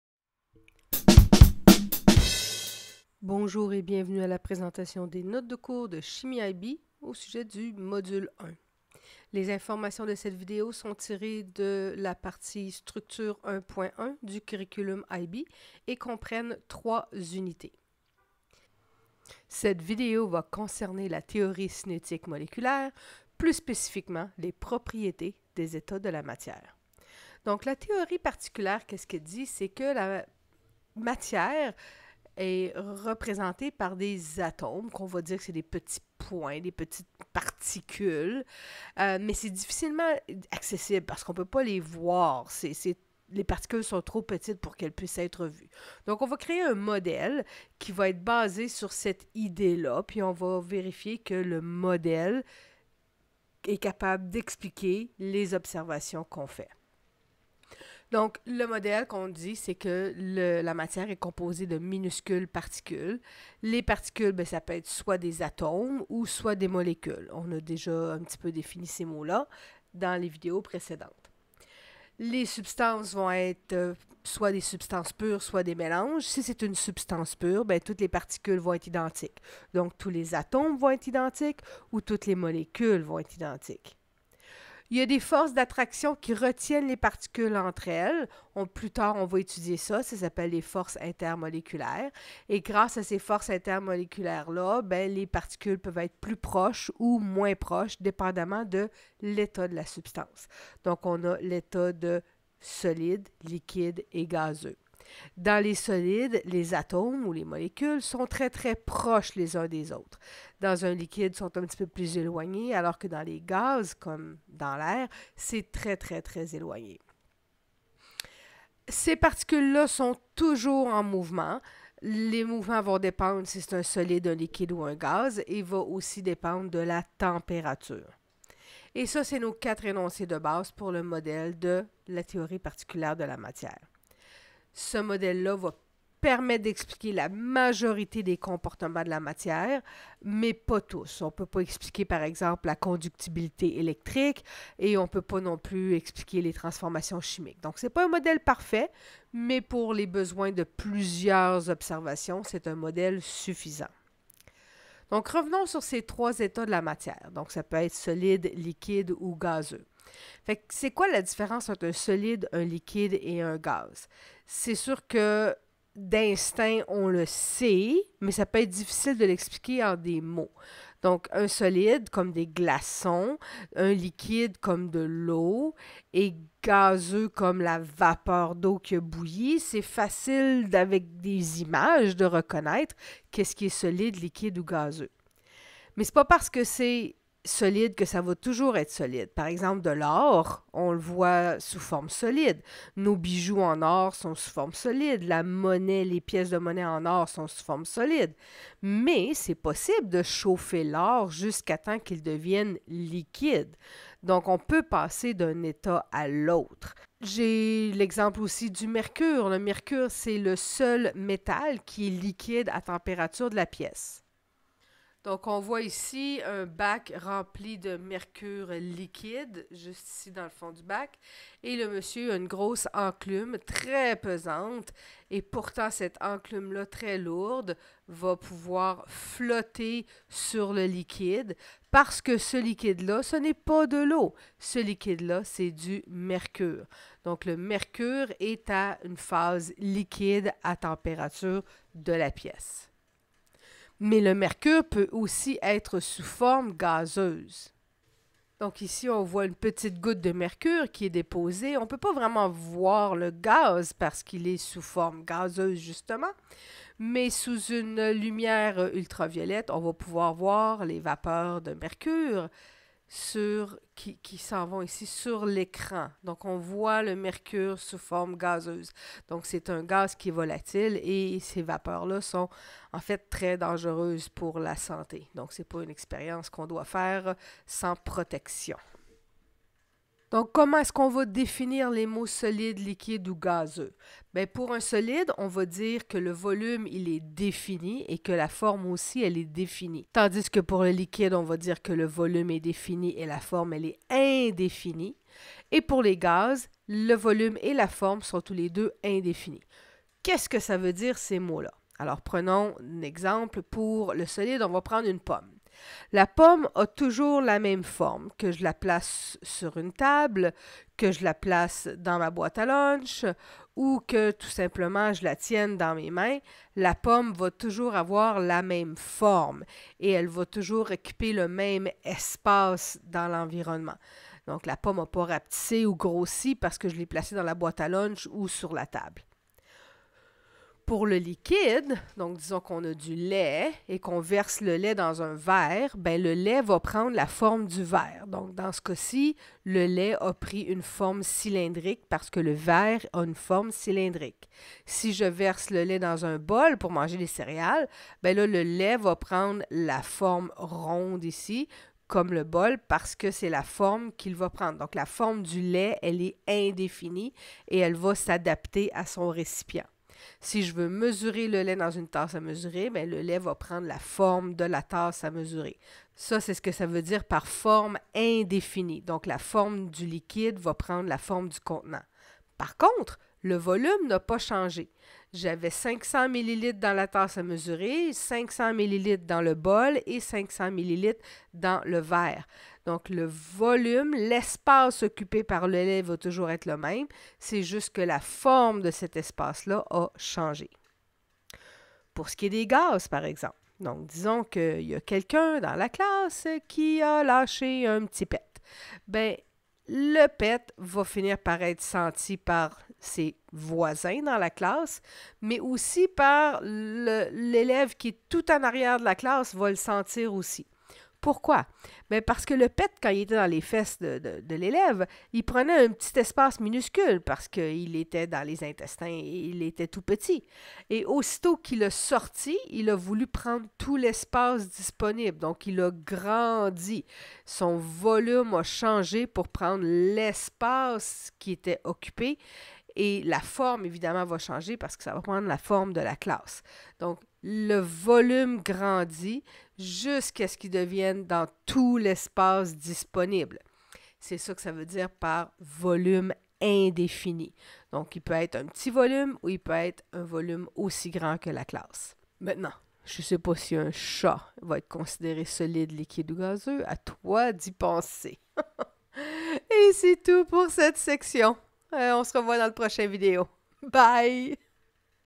Unité 1.2: Les états de la matière PART 1: Propriétés
Chimie IB - Structure 1.1.2 – La théorie cinétique moléculaire est un modèle expliquant les propriétés physiques de la matière (solides, liquides et gaz) et les changements d’état. Distinguer les différents états de la matière.